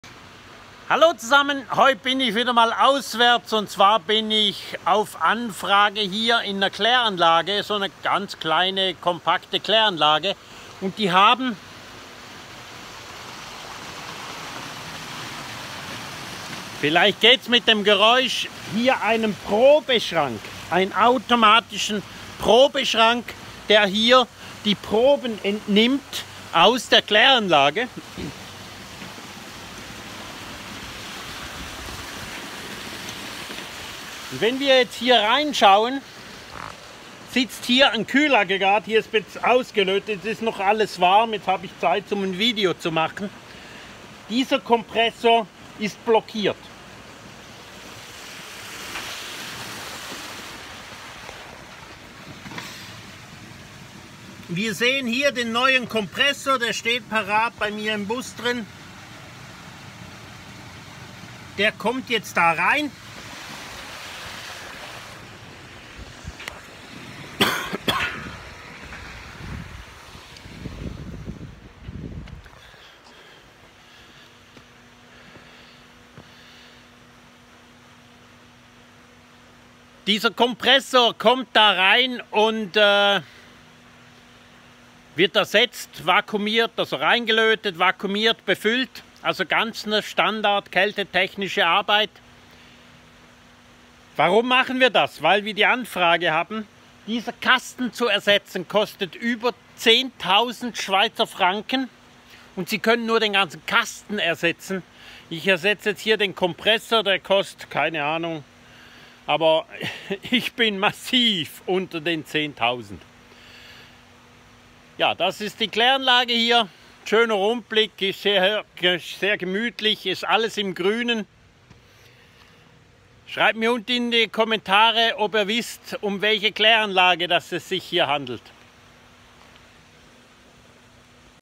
Kompressor wechseln in der Kläranlage Probenentnahme Wasserprobe Kältekompressor Nachhaltigkeit
Kompressor für Wasserproben ersetzt das ist Nachhaltigkeit

► KATALOGE:
Boot und Caravan Kühlgeräte 12 Volt  24 Volt 2024